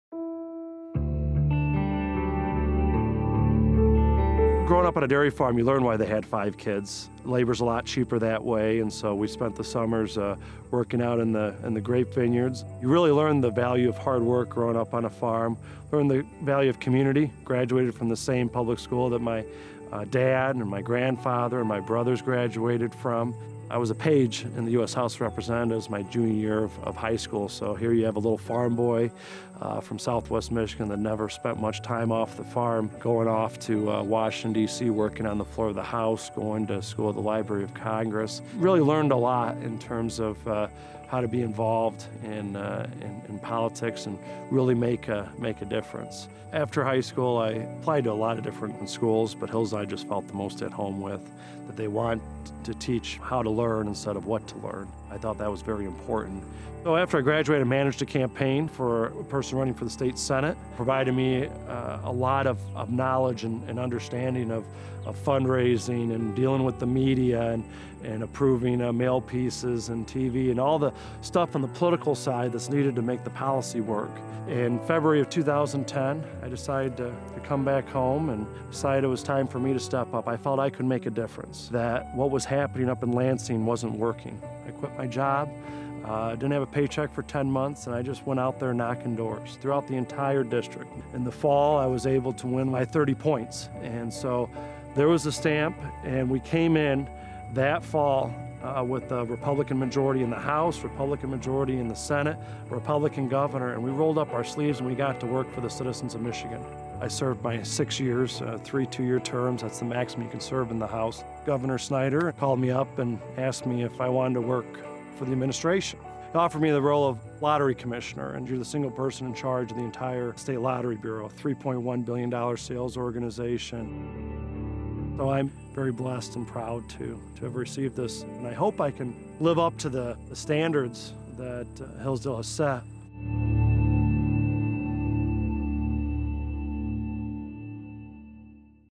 Aric Nesbitt - Distinguished Alumnus Award
Aric Nesbitt became the ninth Commissioner of the Michigan Lottery, and the youngest lottery commissioner in the state’s history, on February 19, 2017. As Commissioner, Nesbitt is responsible for the overall management and operation of the Lottery, including development of long-range plans, policy formulation, assuring the accountability and integrity of the Lottery, and establishing and maintaining contact with state legislators to research, evaluate and gauge the impact of Lottery-related legislation. He previously served in the state House of Representatives from 2011 through 2016. He held several House leadership posts, including Majority Floor Leader and Chair of the Committee on Energy. Prior to winning election to the Michigan House, Nesbitt worked as a legislative director and senior policy advisor for two members of the U.S. House of Representatives (Tim Walberg, R-MI and Tim Murphy R-PA) and also served as a state coalitions manager for a national non-profit organization.
Nesbitt began his career as an aide to a state legislator shortly after graduating from Hillsdale with a bachelor of arts degree in economics. While at Hillsdale, he was involved in Praxis, College Republicans, Student Federation, the International Club, and fundraisers for the Alpha Omega Women’s Care Center. He worked on campus as a computer technician, a driver for CCA speakers, and an assistant in the Admissions Office and at Slayton Arboretum. He also earned a master of science degree in economics and business administration from the Norwegian School of Economics.
Nesbitt grew up on his family’s six-generation dairy farm in Lawton, Michigan, and now lives near the farm with his wife Trisha.